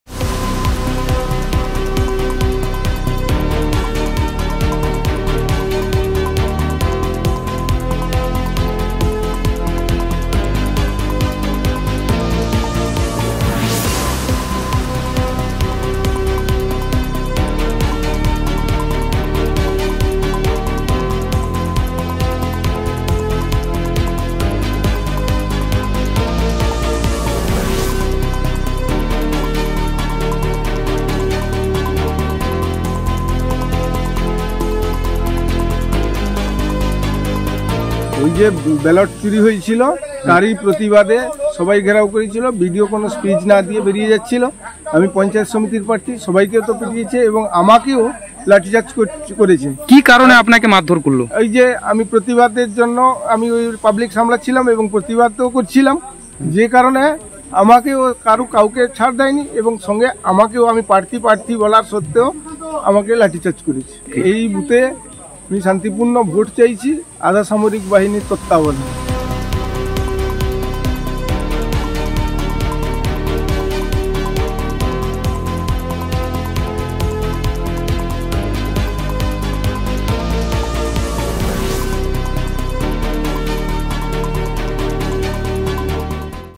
Panchayat Poll: Police chased voters in Purba Medinipur | Sangbad Pratidin
Panchayat Poll: Police chased voters in Purba Medinipur

Patashpur is in unrest due Panchayat Poll. BJP candidates & workers are showing protest in front of the Polling Booth. The police ran after the voters to bring the situation under control. BJP Candidates alleged towards Police Force.